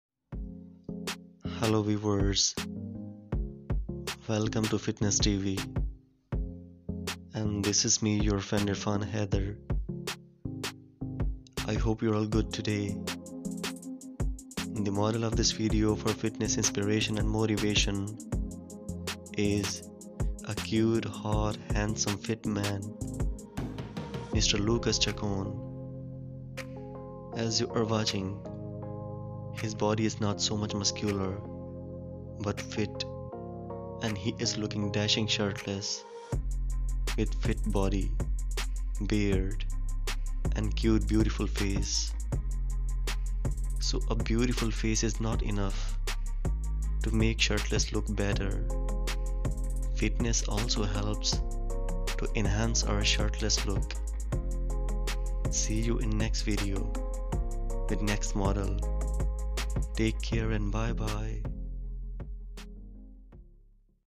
Cute Hot Handsome Man | Lucas Chacon | Fitness
Lucas Chacon fitness reviewed in this video . He is very cute , handsome and hot man with fit good shaped body. He has hairs on his body also but he likes mostly to be shaved body. He is a bearded cute killer muscular man with beautiful attractive smile . He has attractive body with impressive check. He likes swimming . In switched photos he is bathing , swimming and enjoying shirtless and in underwear . His wet hairy body getting attractive because of his fitness and muscles.